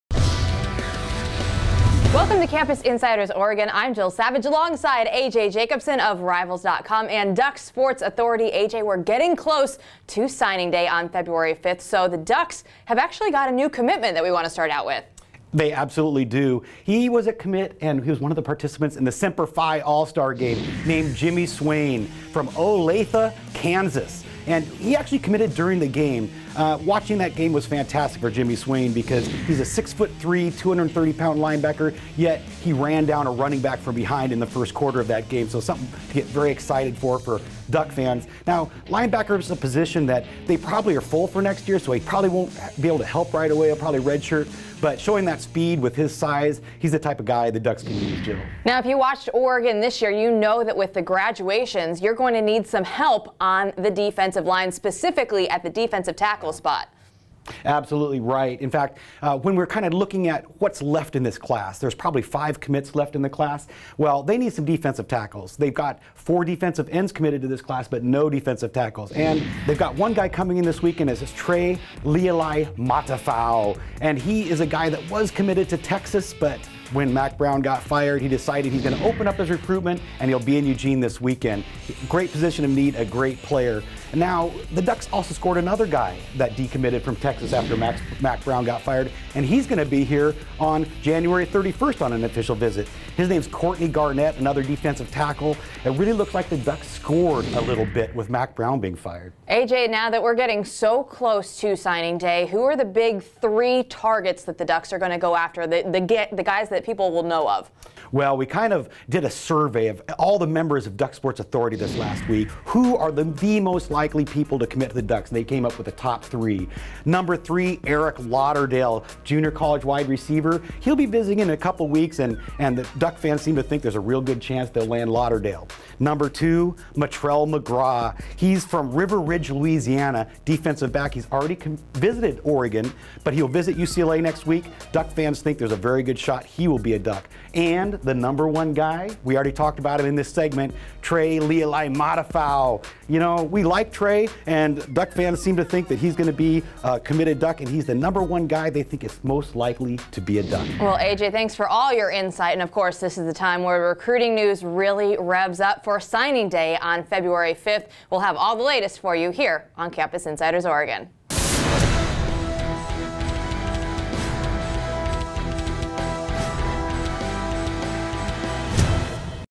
Recruiting Report: Why Mack Brown's Firing May Benefit Oregon | OregonInsiders
Oregon has developed a strong recruiting pipeline to Texas in recent years, and it may have gotten even stronger after Mack Brown was fired.  See who may trade the burnt orange for green and yellow, with just a few weeks to go before National Signing Day.